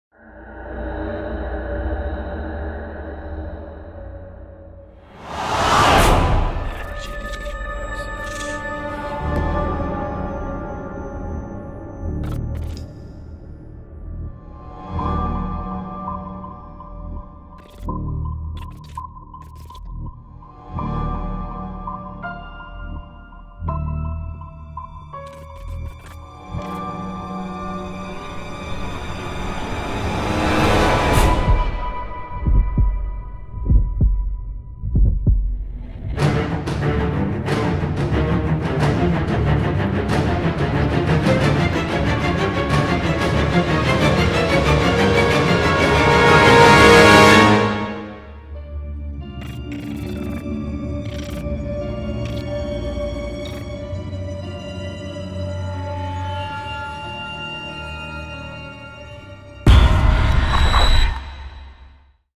Horror places in Kolkata
we have tried to cover then main horror places in kolkata and made a special trailer to show a glimpse of horror in kolkata...